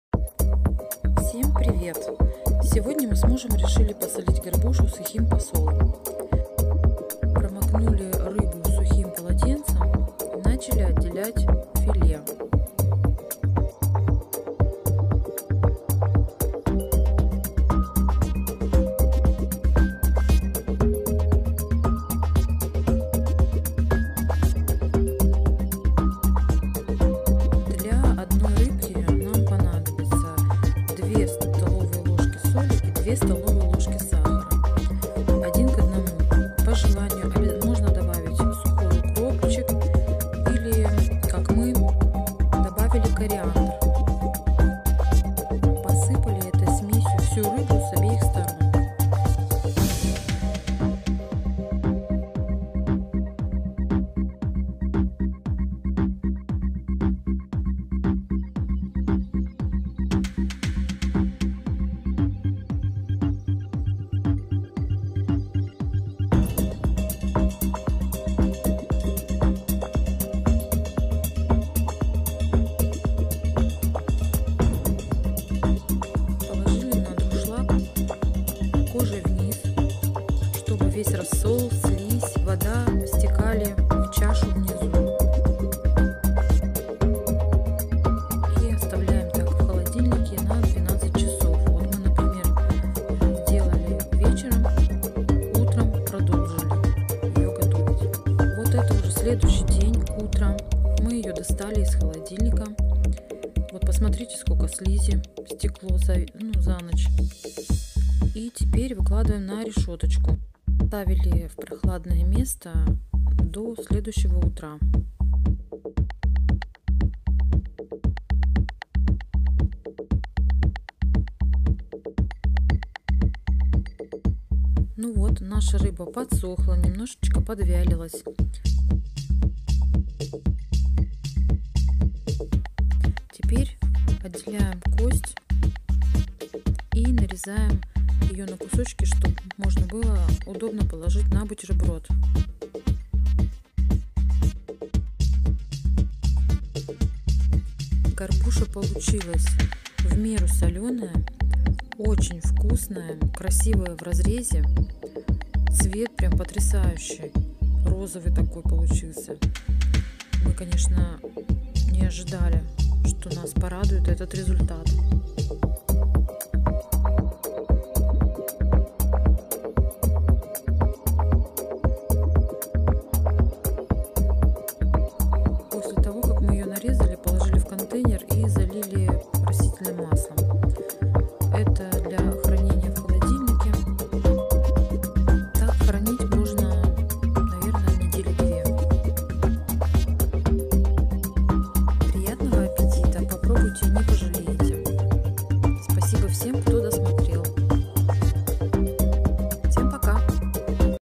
ГОРБУША вкусного, сухого посола. МУЖ доволен!!!рецепт
Ингредиенты
На одну
2 стл соли
2 стл сахар
Сухой укроп или кориандр по вкусу.